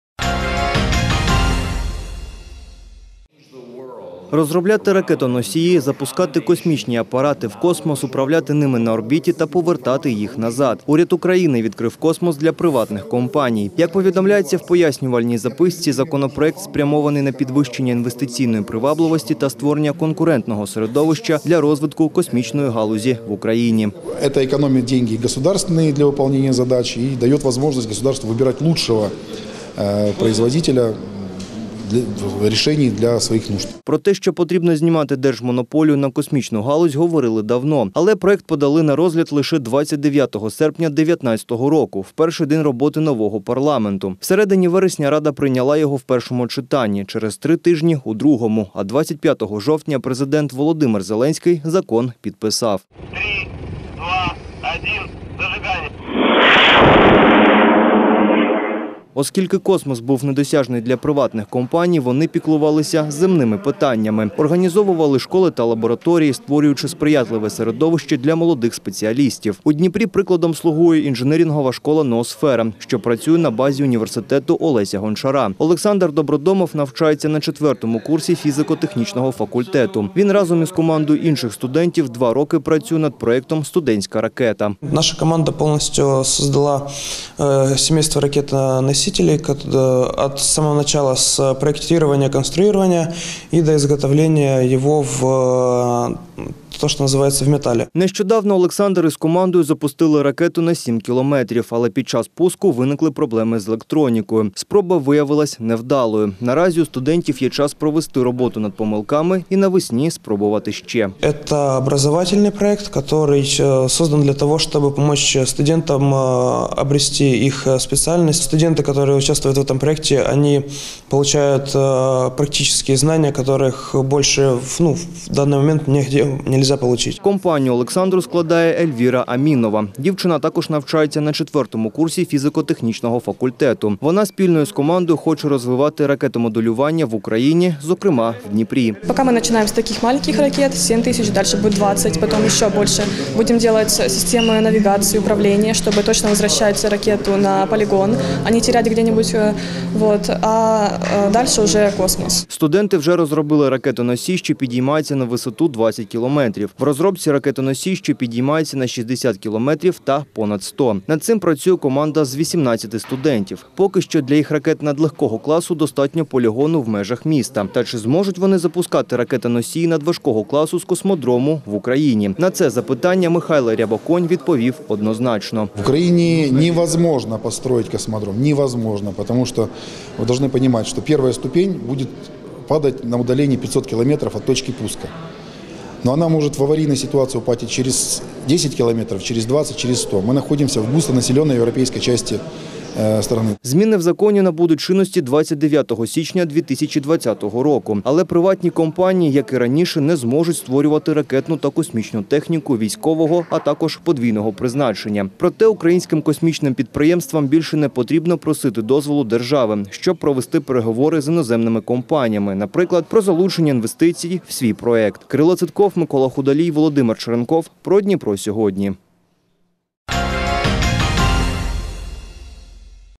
Уряд України відкрив космос для приватних компаній
Розробляти ракетоносії, запускати космічні апарати в космос, управляти ними на орбіті та повертати їх назад. Уряд України відкрив космос для приватних компаній. Як повідомляється в пояснювальні записці, законопроект спрямований на підвищення інвестиційної привабливості та створення конкурентного середовища для розвитку космічної галузі в Україні.